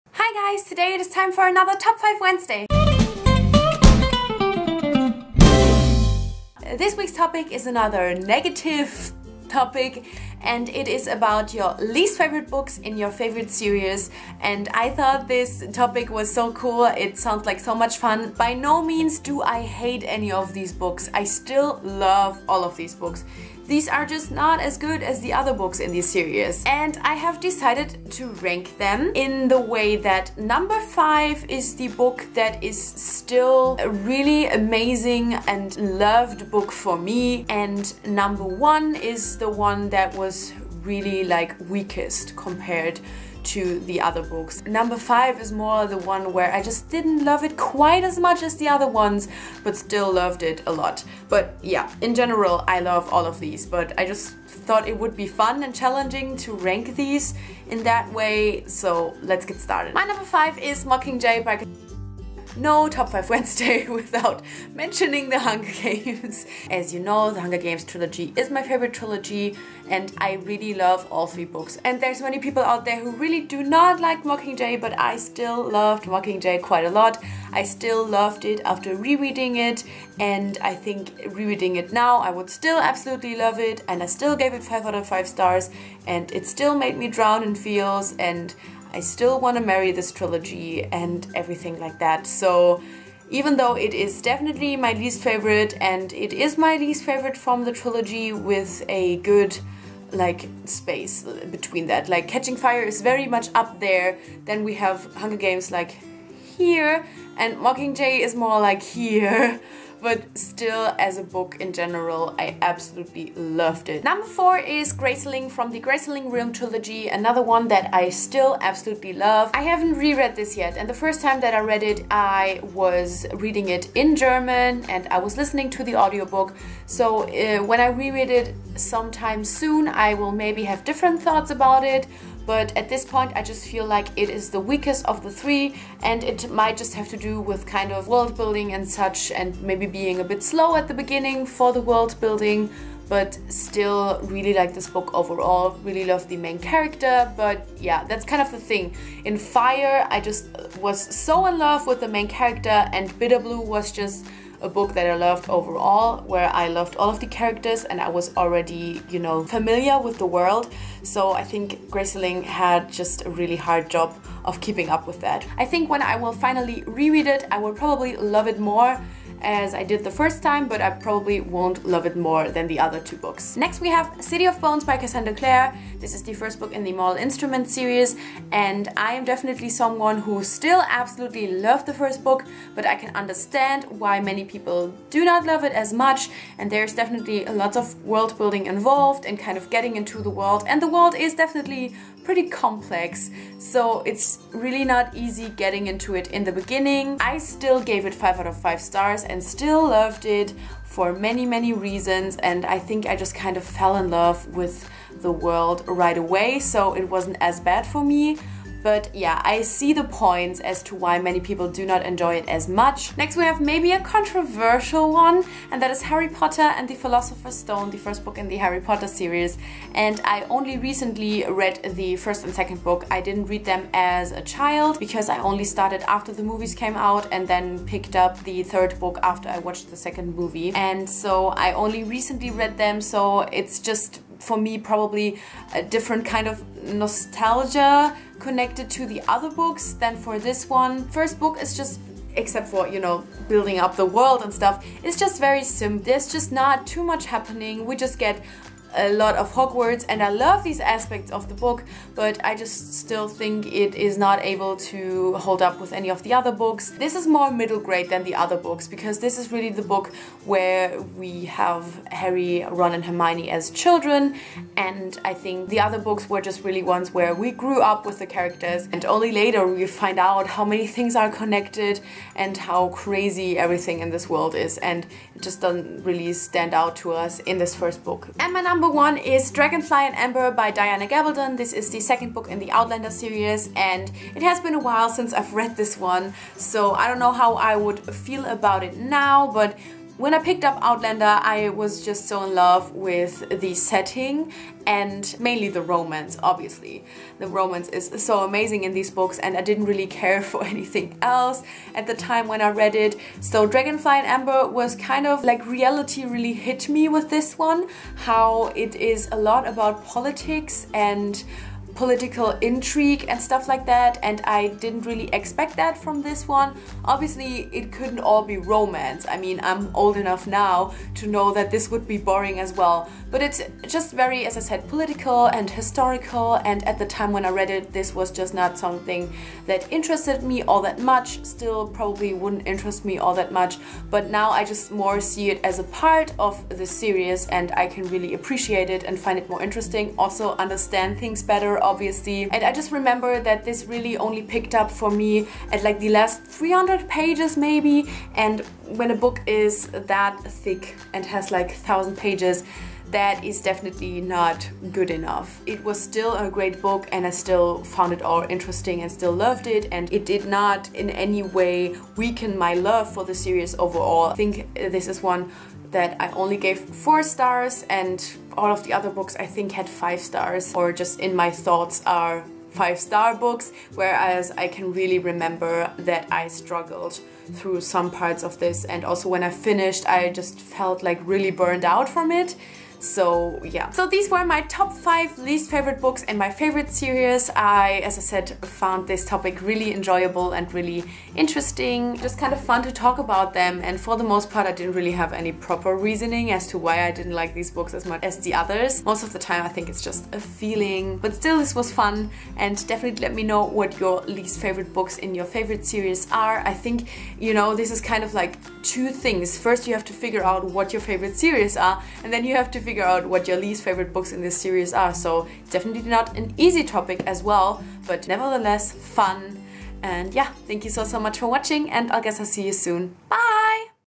Least Favourite Books in My Favourite Series | Top 5 Wednesday
Today it's time for another Top 5 Wednesday, this time on my least favourite books in my favourite series, one of the coolest topics yet!